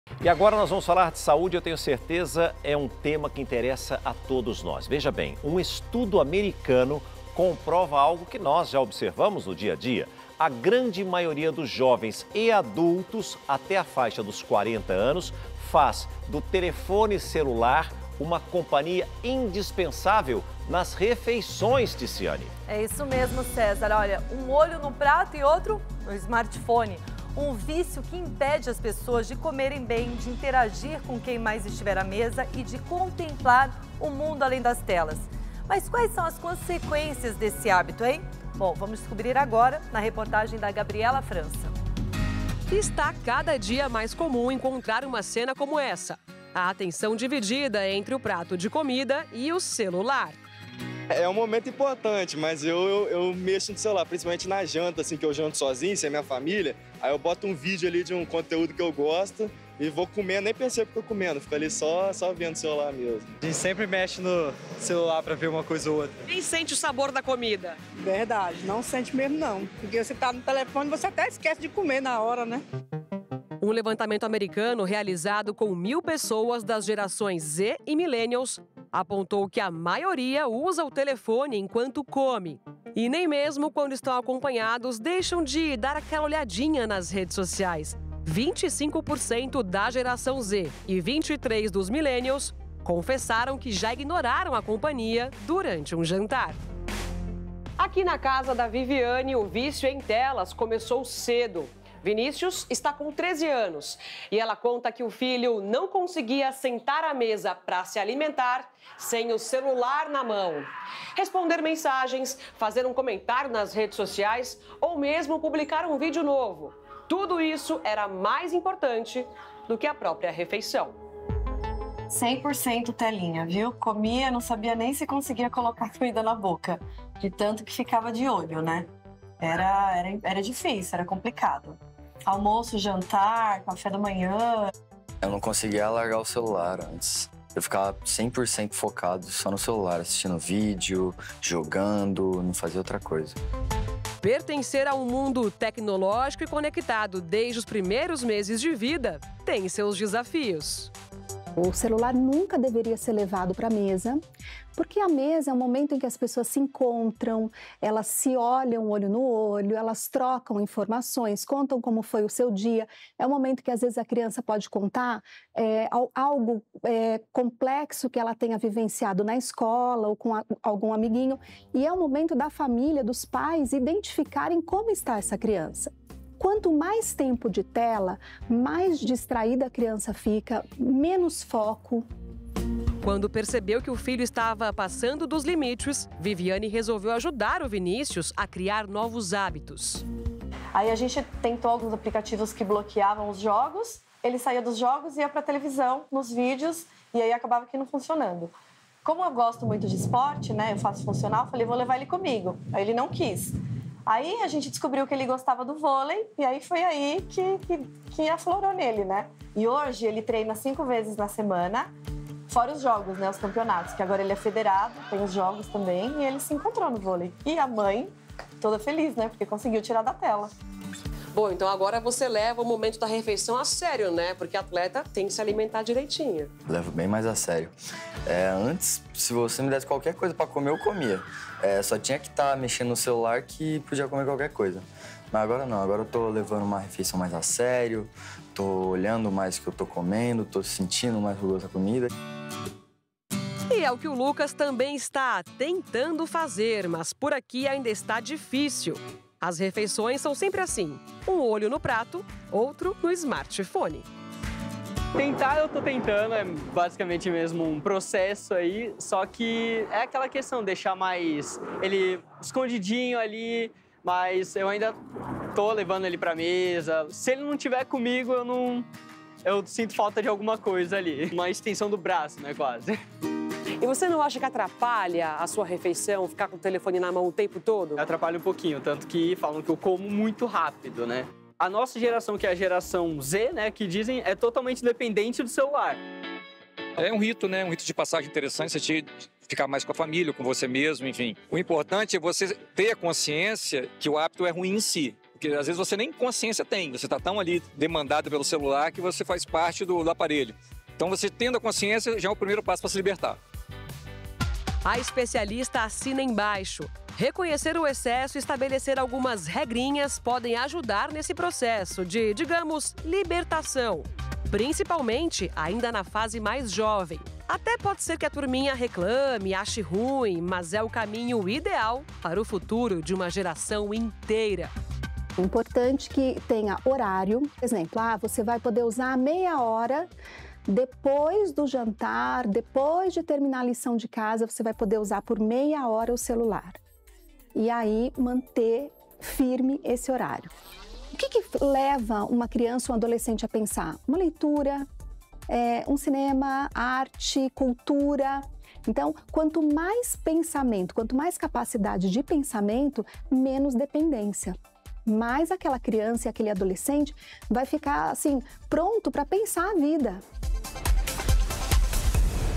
Vício em celulares prejudica qualidade da alimentação
Um estudo americano comprova algo que nós já observamos no dia a dia. A grande maioria dos jovens e adultos até a faixa dos 40 anos faz do telefone celular uma companhia indispensável nas refeições. Um vício que impede as pessoas de comer bem, de interagir com quem mais estivar à mesa e de contemplar o mundo além das telas.